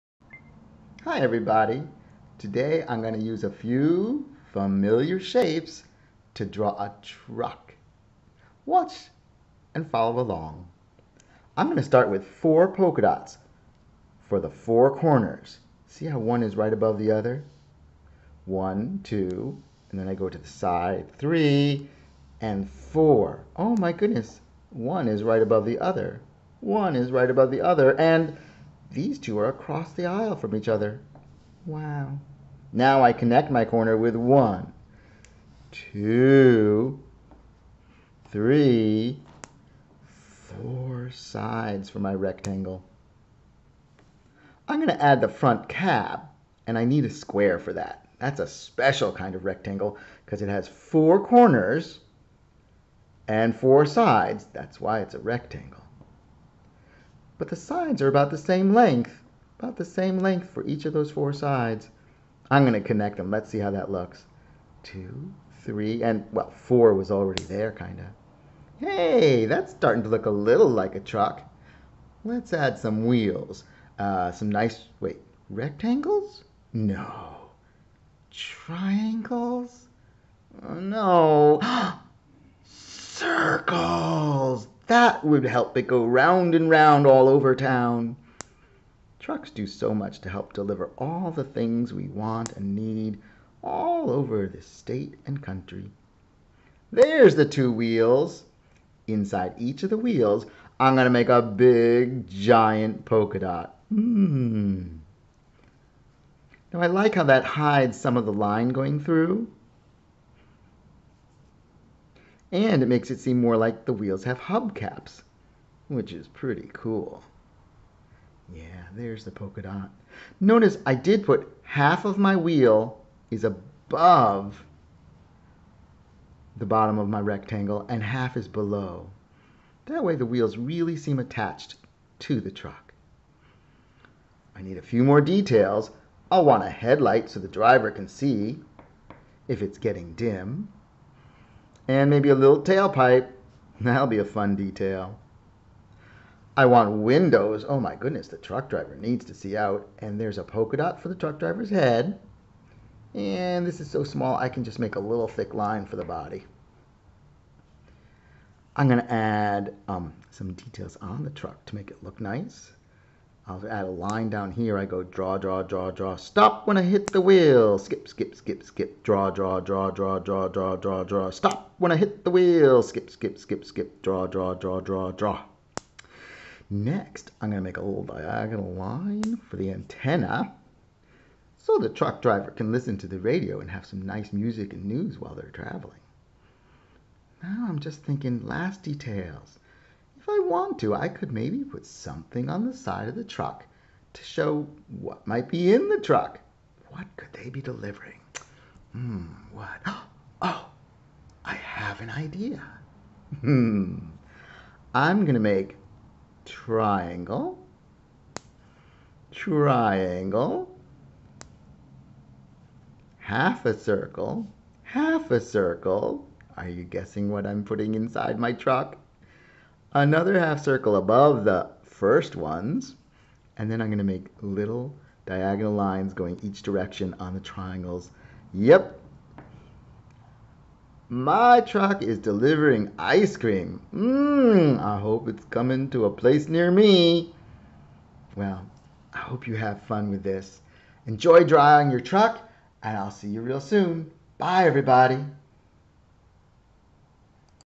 Draw a Truck with Rectangles, Circles, and Triangles!
Most things in the world can be drawn with a few, familiar shapes. Draw a truck with Mr. RB as he uses rectangles, circles, and triangles to get this drawing "on the road"!.